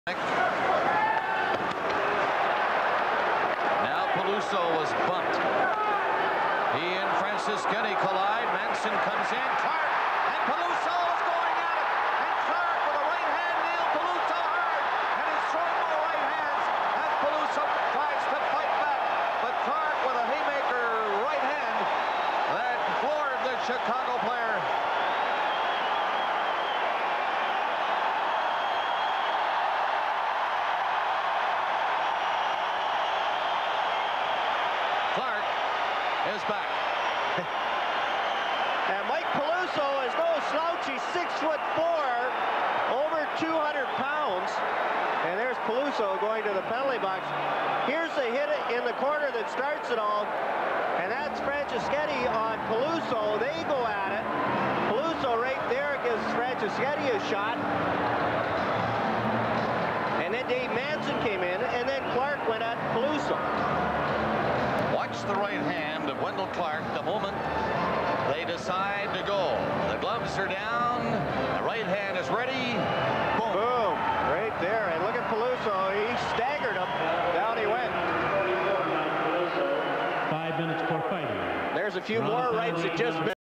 Wendel Clark TKO's Mike Peluso Round 2
Wendel Clark ( Toronto Maple Leafs ) knocks down Mike Peluso ( Chicago Blackhawks ) - NHL hockey fight Oct 20, 1990